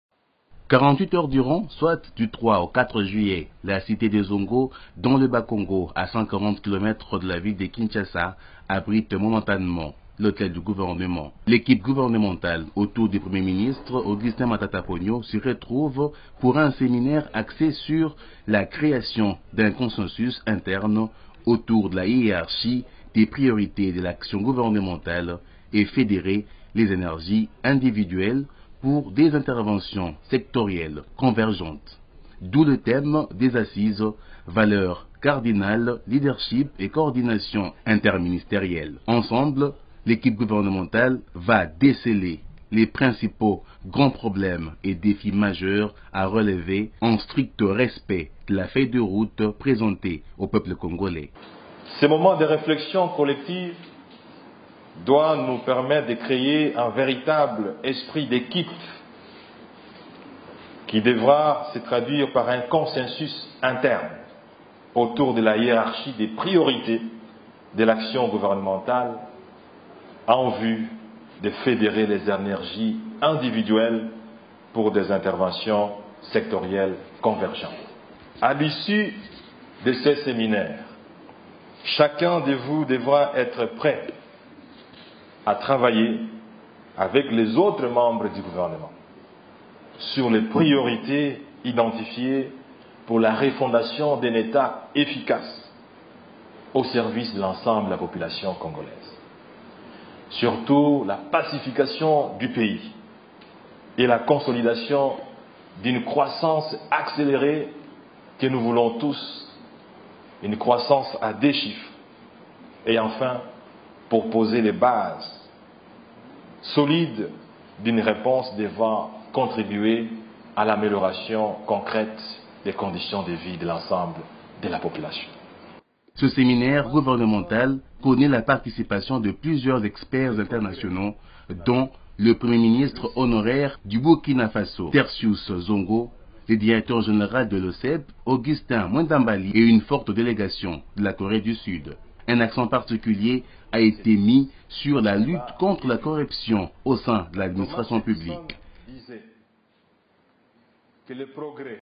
Le gouvernement Matata en conclave à Zongo pour peaufiner ses stratégies de conduite...
Le gouvernement Matata en conclave à Zongo pour peaufiner ses stratégies de conduite de son programme d'action